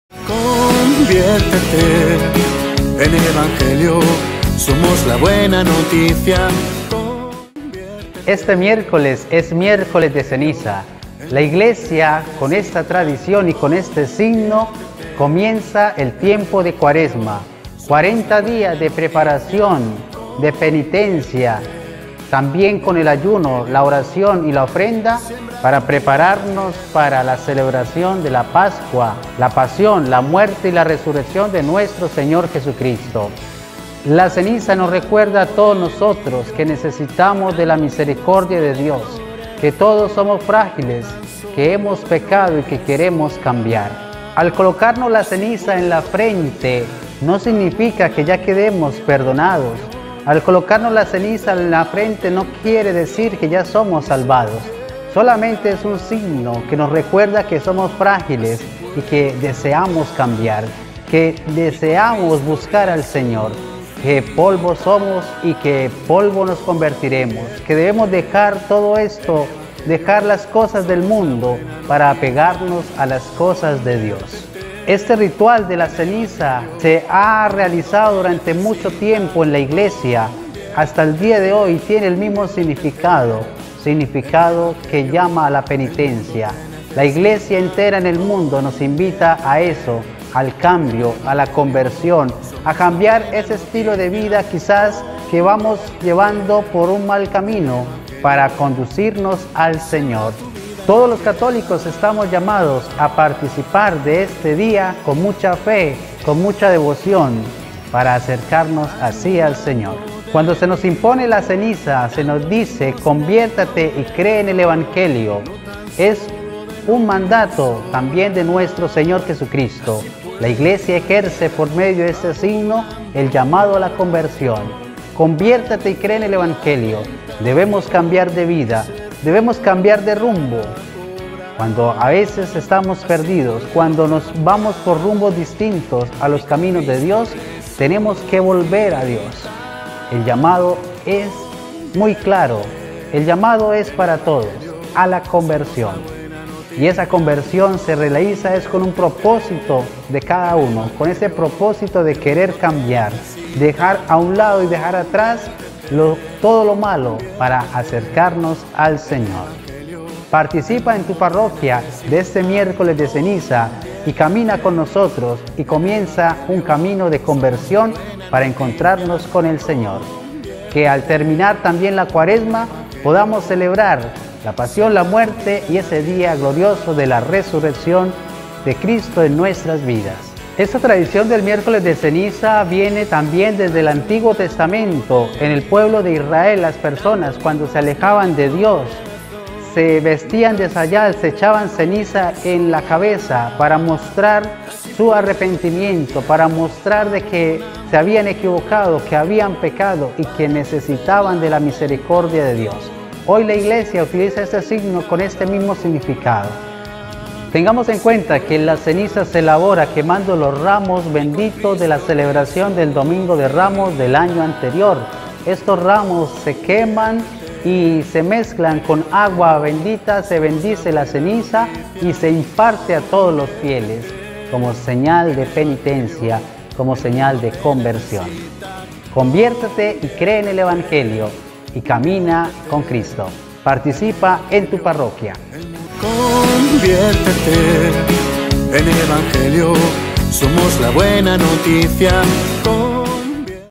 Miércoles de Ceniza 2022
✅La palabra ceniza, que proviene del latín "cinis", representa el producto de la combustión de algo por el fuego. Esta adoptó tempranamente un sentido simbólico de muerte, caducidad, pero también de humildad y penitencia.

La ceniza, como signo de humildad, le recuerda al cristiano su origen y su fin: "Dios formó al hombre con polvo de la tierra" (Gn 2,7); "hasta que vuelvas a la tierra, pues de ella fuiste hecho" (Gn 3,19).